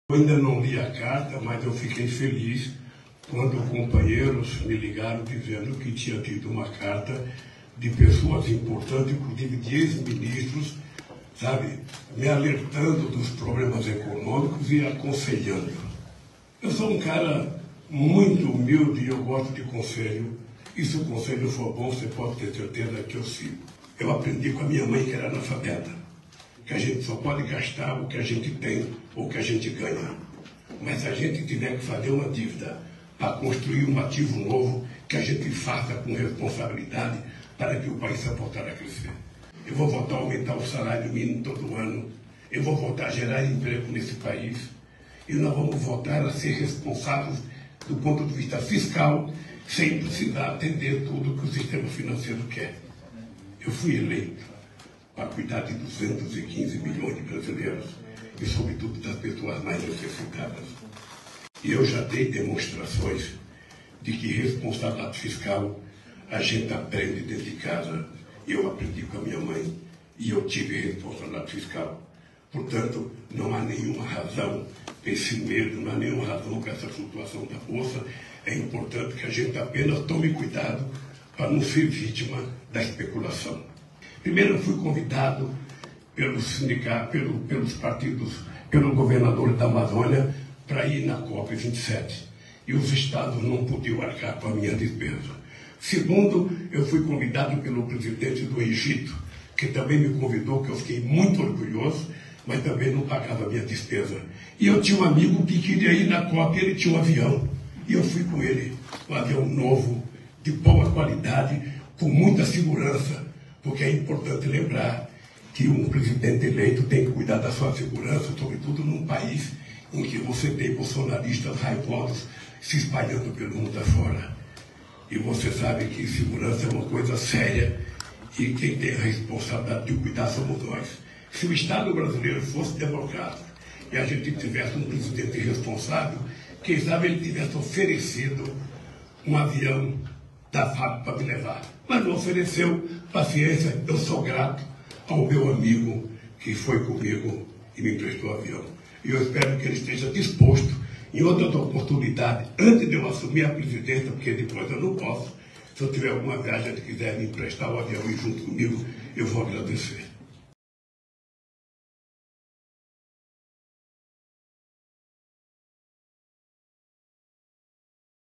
'Se o conselho for bom, eu sigo', diz Lula sobre carta de alerta fiscal de economistas
Após encontro com o primeiro-ministro de Portugal, António Costa, o presidente eleito garantiu que novo governo terá responsabilidade fiscal e comentou sobre viagem à COP em avião de empresário.